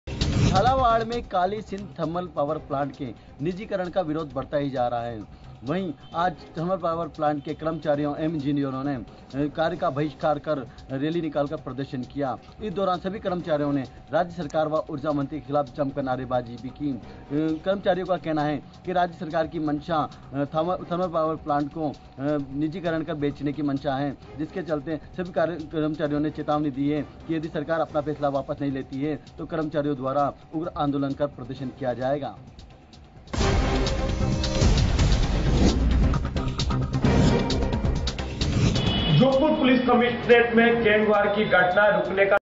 झालावाड़ थर्मल पावर प्लान्ट निजीकरण के विरोध में रैली Protest privatization thermal power plant
झालावाड़ में थर्मल पावर प्लान्ट के निजीकरण के विरोध में रैली Rally to protest privatization thermal power plants in Jhalawar